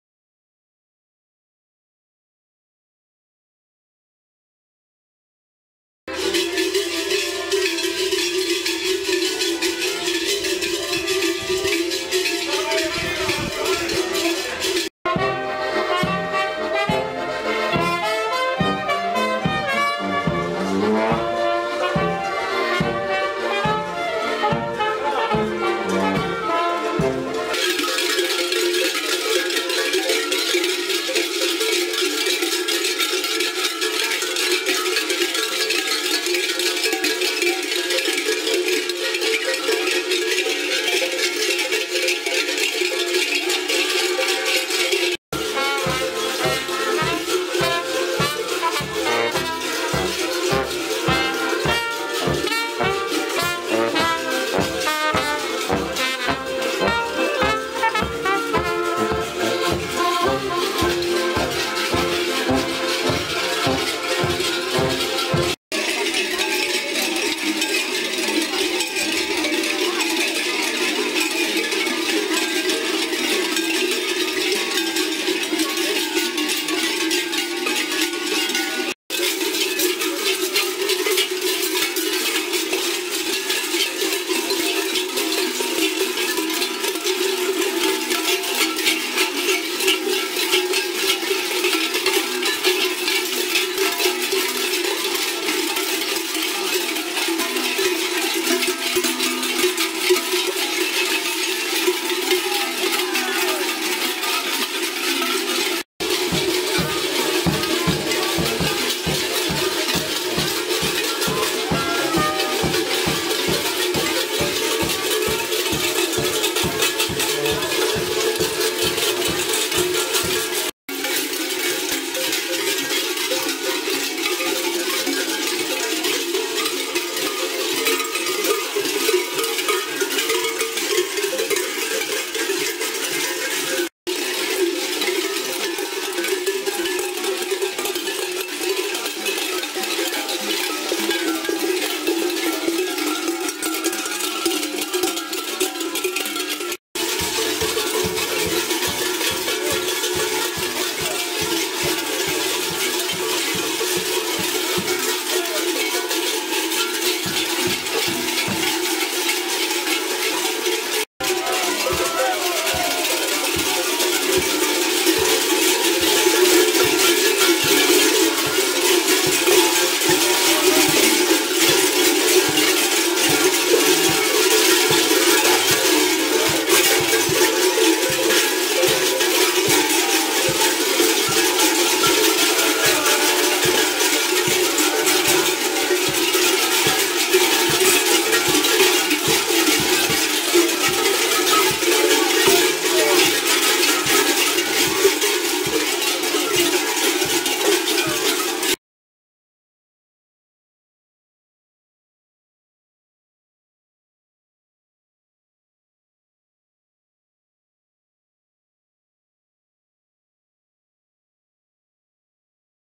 Zvonejski zvončari storili užancu i odradili uzvratni pohod Rukavčanon @ Zvoneća 2020.
Zvonejski zvončari storili užancu i odradili uzvratni pohod Rukavčanon @ Zvoneća